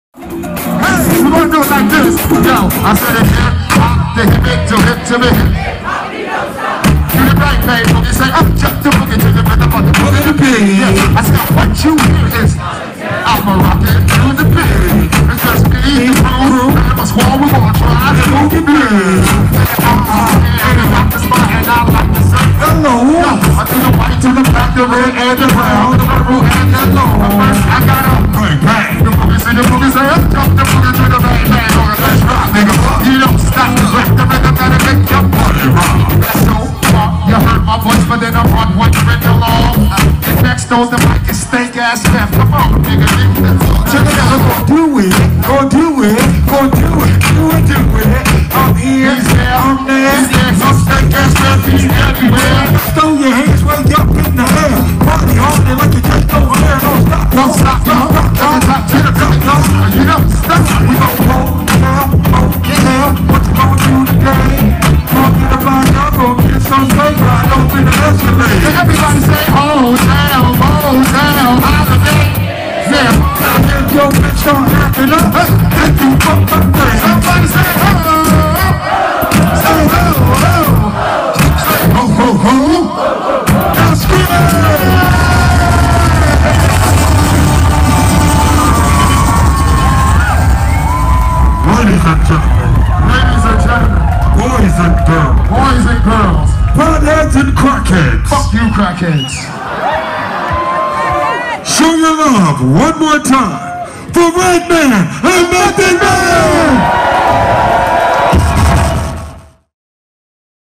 Redman & Method Man Perform @ the Fire Bowl
POT TV - WATCH Redman & Method man perform LIVE @ the Canadian Fire Bowl.

The Canadian Fire Bowl is a cannabis strain testing competition, that took place in Vancouver, BC.